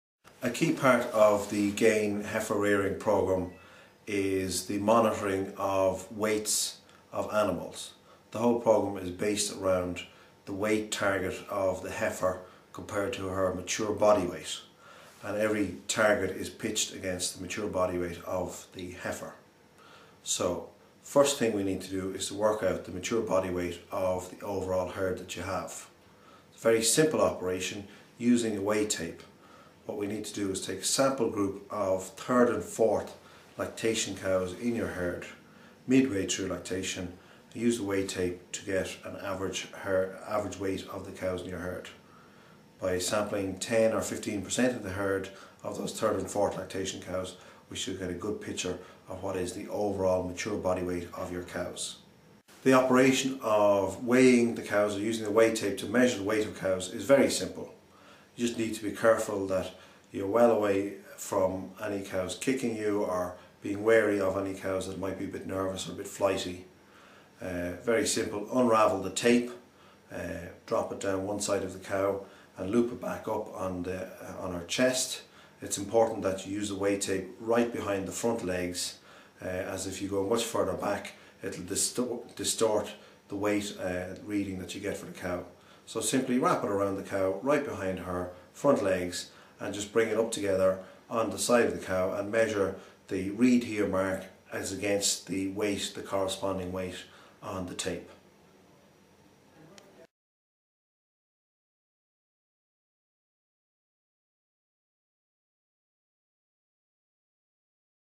Cow Weight Tape Demo - Gain Feeds Heifer Rearing Programme
The Gain® Heifer Rearing Programme brought to you from Glanbia and Alltech is specifically designed to help farmers to maximise profitability by calving heifers at their target weight at 24 months.This is an integrated 2 year
programme that breaks the heifer's life into 5 distinct phases where each receives a special focus in order to increase the chances of success.

How to set targets:
As all of the weight targets for the programme are set as a percentage of the mature body weight it is essential to get a mature weight of your herd to use as a starting point. In order to do this you need to use the weight tape, or weighing scales if you have access to them, to measure your cows. Choose a sample group of 3rd and 4th lactation cows mid-way through their lactation. Record the weight and work out the average mature weight of your herd and all targets will be set from this figure.

Monitoring weight targets:
Every farmer should have a weight measuring tape so that heifers can easily be measured and managed. It is a very simple operation to carry out and should be done every few months or when heifers are brought in for handling i.e. dosing, TB testing or vaccinations. Weights should be recorded and comparisons made against targets.

-Target of 60% of mature body weight at breeding
-Target of 15 months breeding age
-Calve heifers at 90% of Mature Body Weight (MBW)
-Calve heifers down at BCS of 3.0